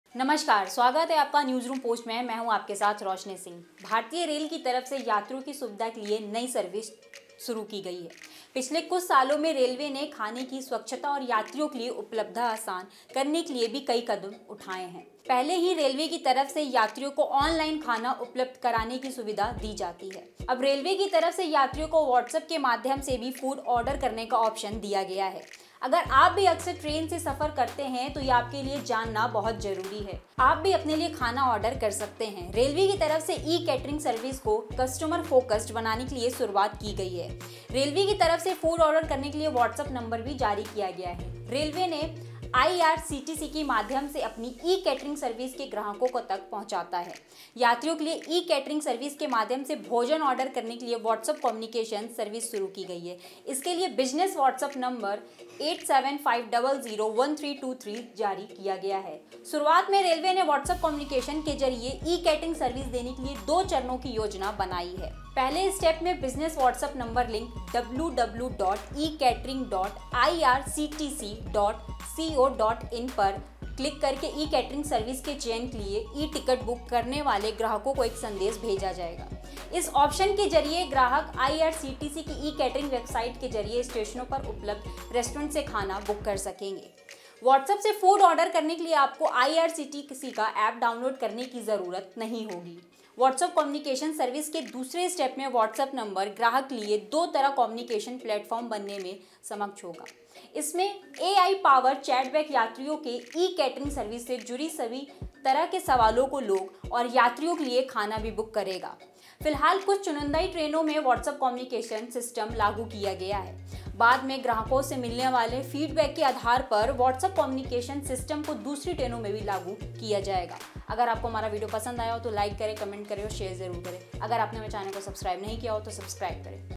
Indian Railways | ट्रेन में वॉट्सअप से भी फूड ऑर्डर कर सकेंगे यात्री , रेलवे ने जारी किया नंबर
Indian Railways | New services are being started by the Indian Railways for the convenience of the passengers. In the last few years, Railways has also taken several steps to improve the hygiene of food and ease of availability for passengers. Already, the facility of providing online food to the passengers is provided by the Railways. Now the option of ordering food through WhatsApp has been given to the passengers by the Railways. If you also often travel by train, then after reading this news, you can also order food for yourself.